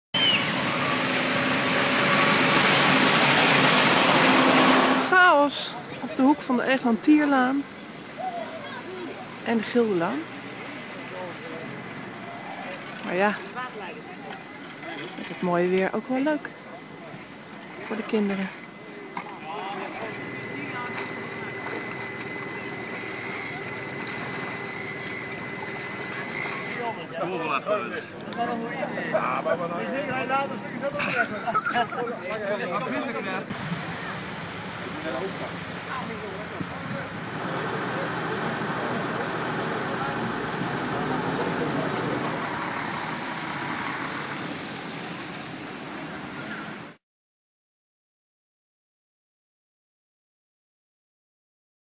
Wateroverlast Apeldoorn
Het knappen van een 30 centimeter dikke waterleiding heeft vanmiddag voor chaos gezorgd in de Apeldoornse wijk De Maten.